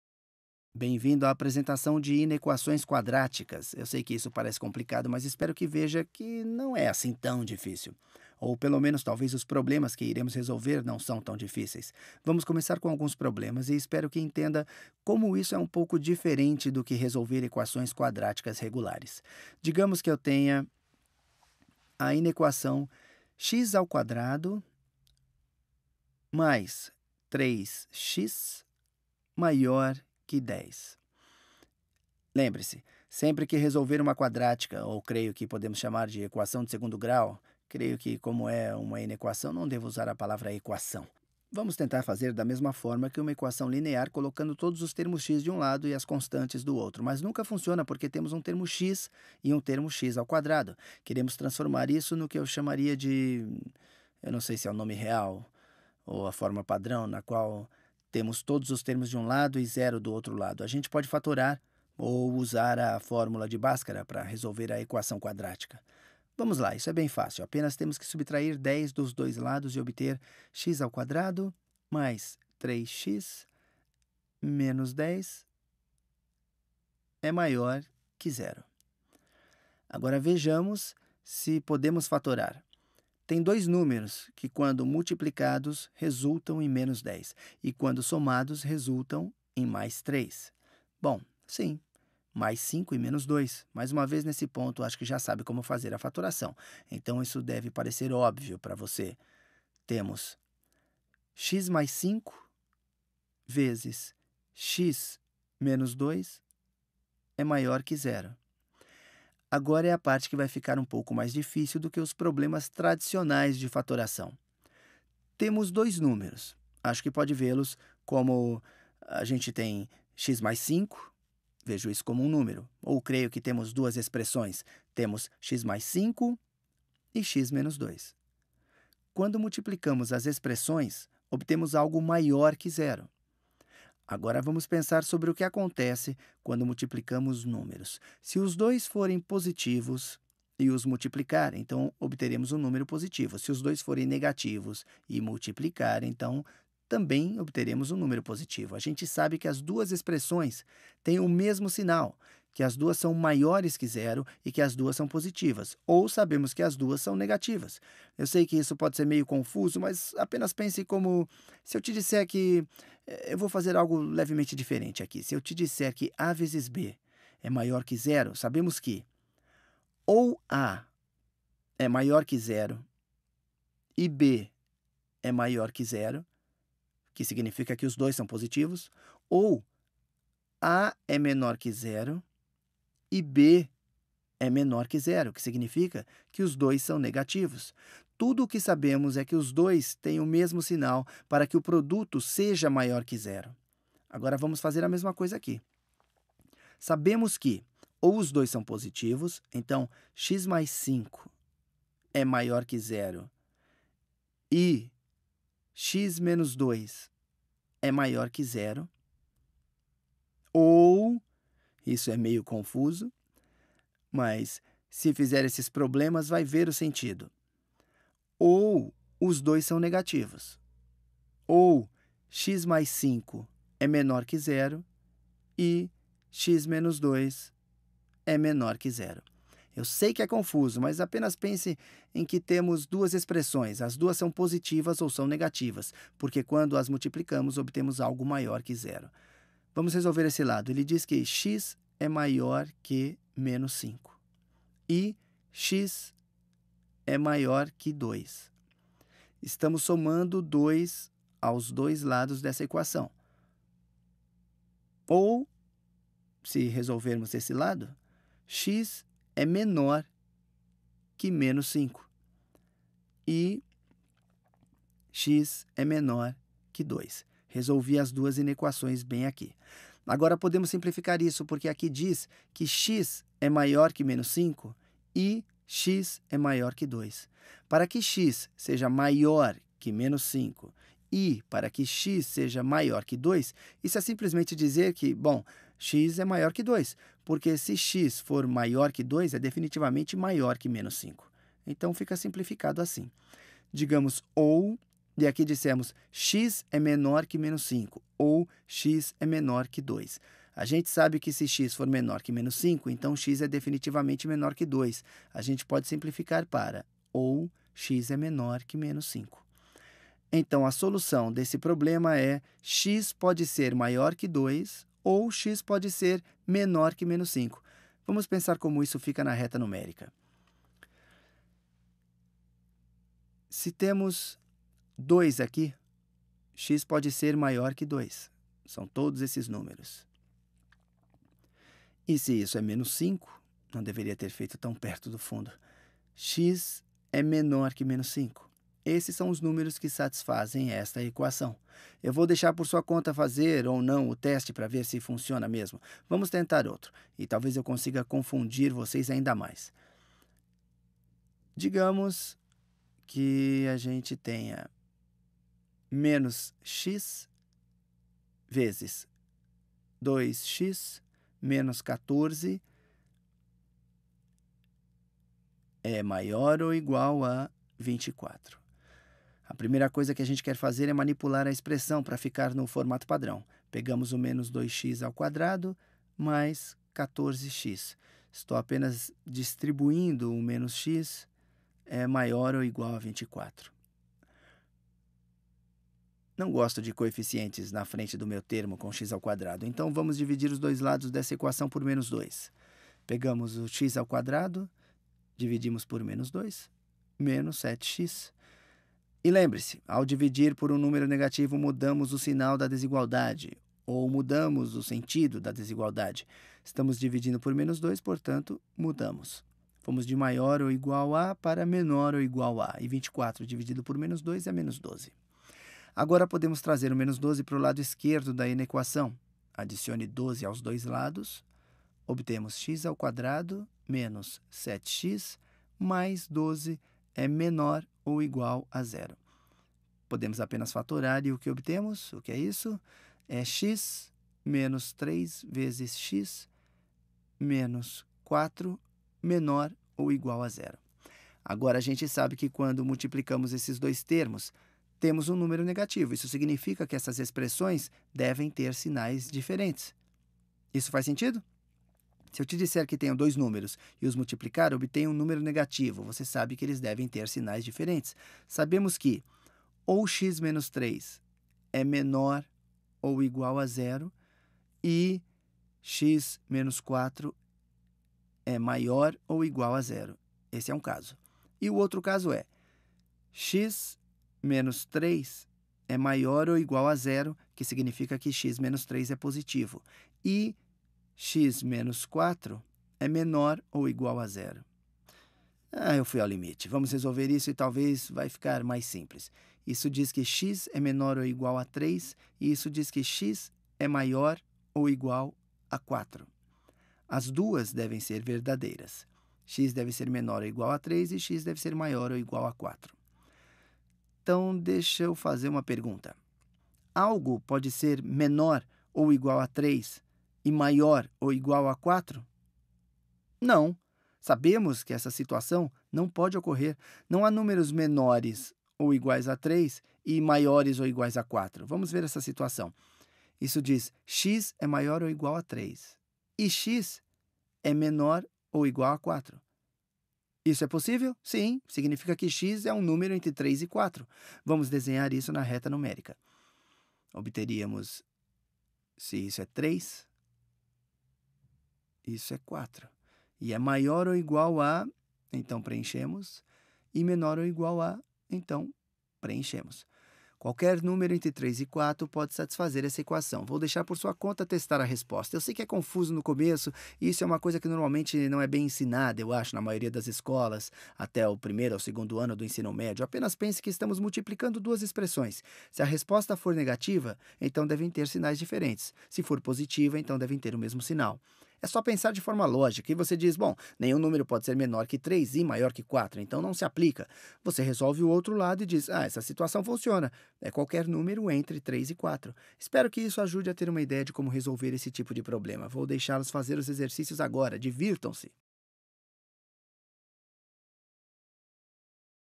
Inequações Quadráticas
Vídeo sobre resolução de inequações quadráticas ou inequações do segundo grau.
Titulo em inglês: Quadratic Inequalities